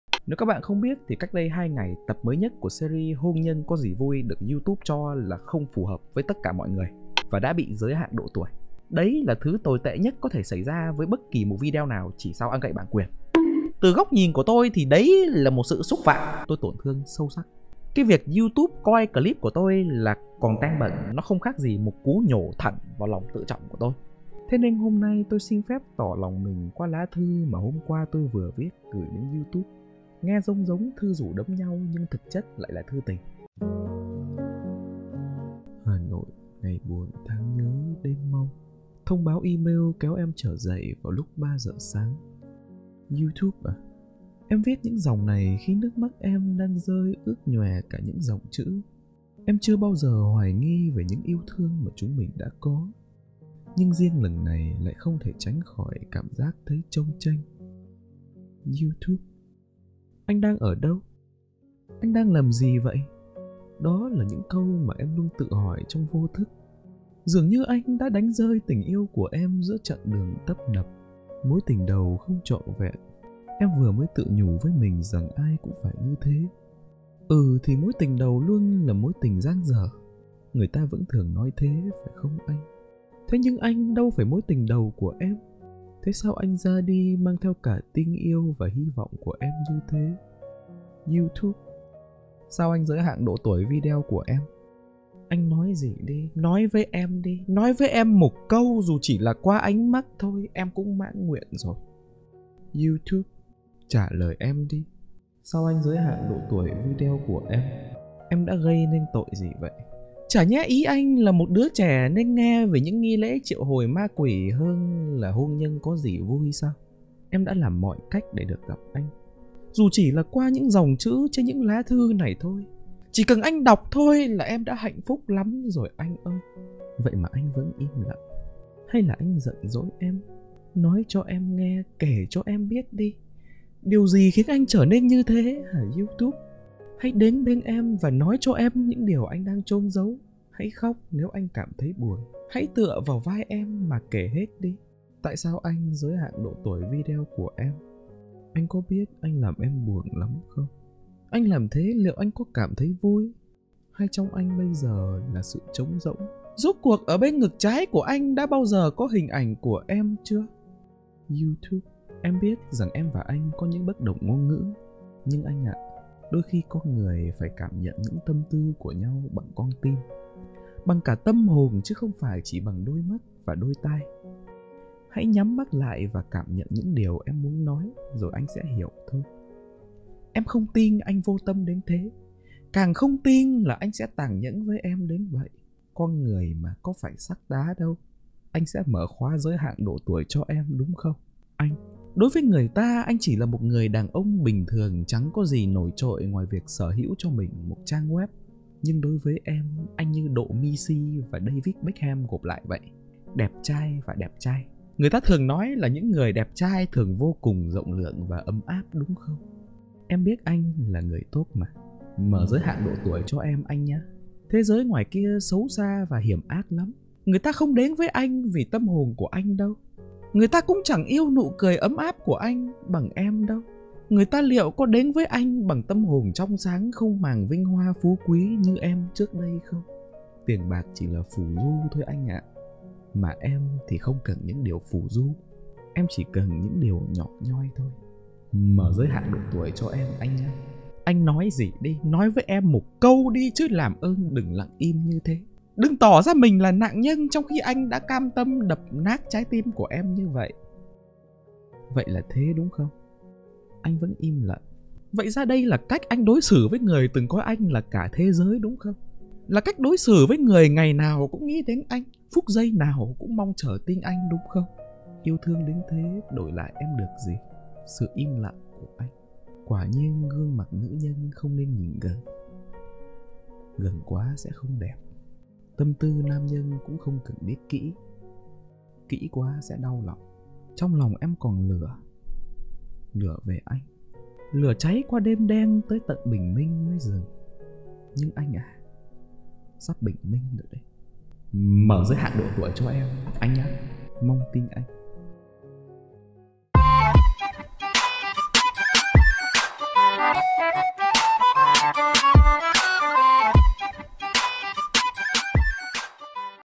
Thư Tình Tuna Gửi Youtube | Tại Sao Lại Cấm "Hôn Nhân Có Gì Vui"
Đừng ngại gợi ý game ở dưới comment
Chúc anh em một ngày tốt lành
Nội dung lấy cảm hứng lớn từ fanpage Chửi Thuê